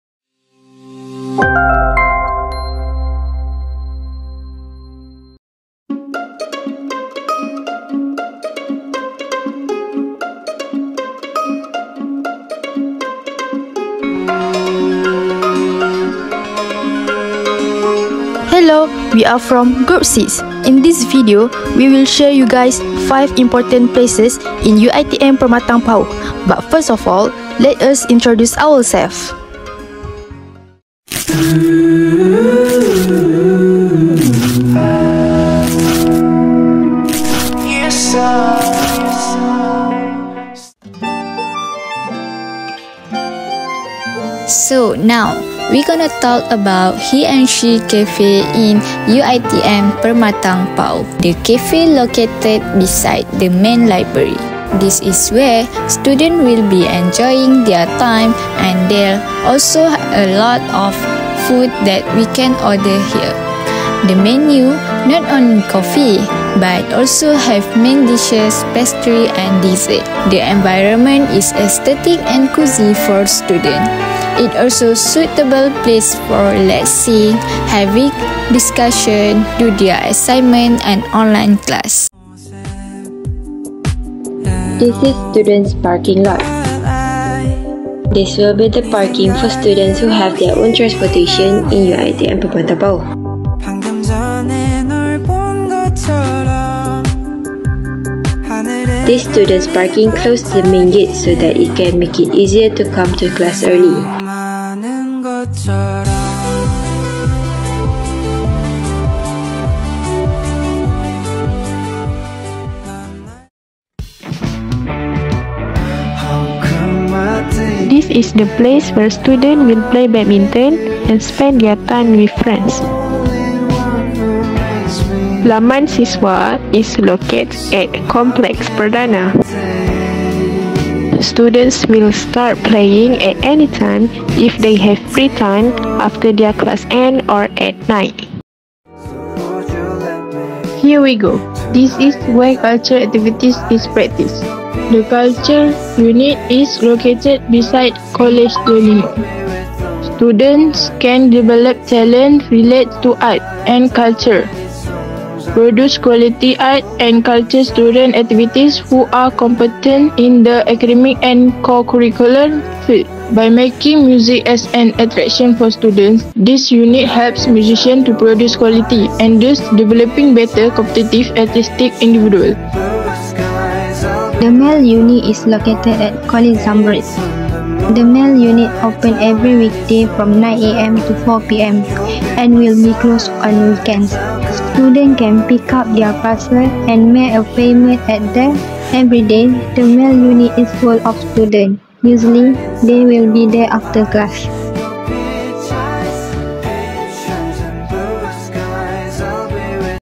UED 102 || 5 IMPORTANT PLACES IN UiTM PERMATANG PAUH PULAU PINANG
Assalamualaikum and hi !
This is our short video about 5 important places in UiTM Permatang Pauh.

- He & She Coffee Cafe
- Students Parking
- Laman Siswa
- Cultural Unit
- Mail Unit

KUMPULAN 6

NUR DAMIA QAISHARA BINTI SHAHARUDDIN

NUR HANIS FATIHAH BINTI ABDUL HAKIM

NUR ADRIANA KAMILIA BINTI MOHD AZAM

FARAH FAZIDATUL AKMAL BINTI MOHD IKRAM

NUR ALEYA FARISHA BT MOHD NOOR